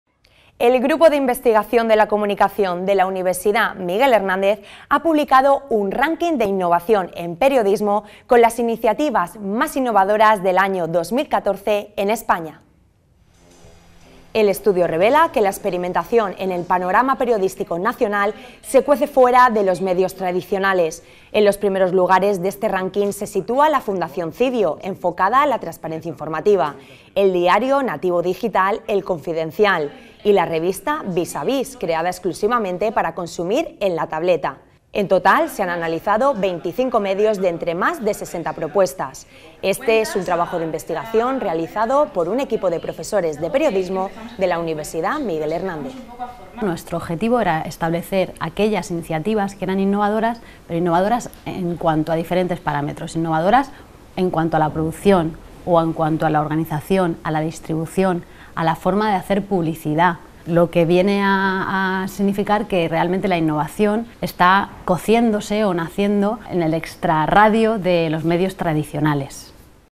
UMH TV - Ranking 2014 de Innovación Periodística de la UMH
El Grupo de Investigación de la Comunicación de la Universidad Miguel Hernández ha publicado un ranking de innovación en Periodismo con las iniciativas periodísticas más innovadoras del año 2014 en España.